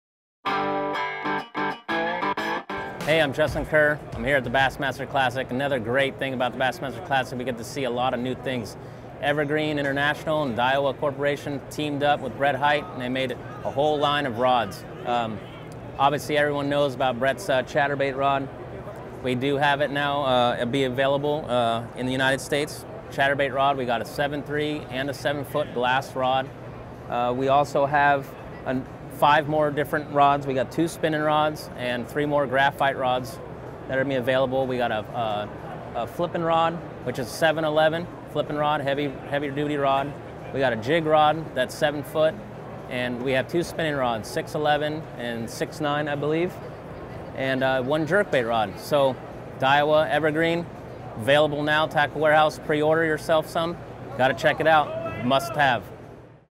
Evergreen Brett Hite Combat Stick Rods with Justin Kerr | Bassmaster Classic 2017
Evergreen Brett Hite Combat Stick Rods with Justin Kerr at the 2017 Bassmaster Classic Expo.